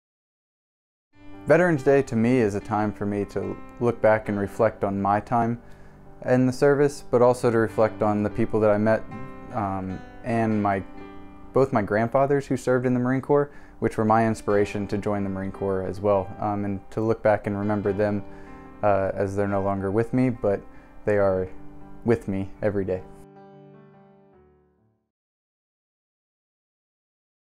FCPS Veteran - Brandon Filsinger
Brandon Filsinger, a teacher at Kilmer Middle School, served our country in the Marine Corps.